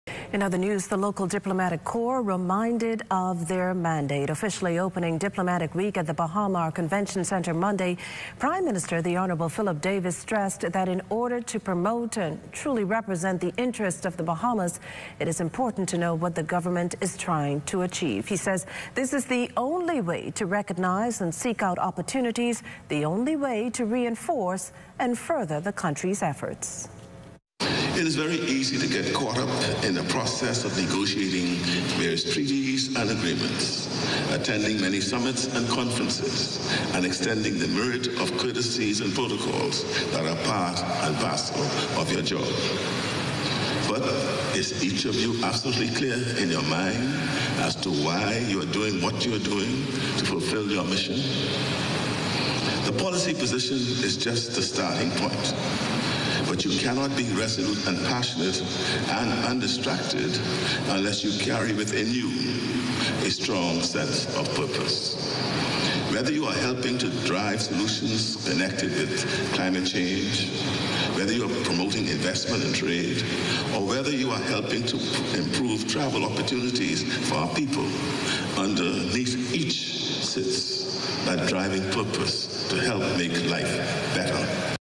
PM Officially Opens Diplomatic Week
Prime Minister, the Hon. Philip Davis brings remarks at the opening of diplomatic week.